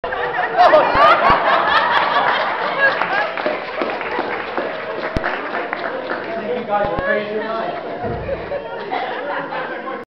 David Klinedinst veszprem running hugs.avi
During my talking on family worship, I shared how I do "running hugs" with my girls before bedtime.  They didn't know what that was.  So I demonstrated by telling the translator to pretend he was 5.  He actually did a running hug with me!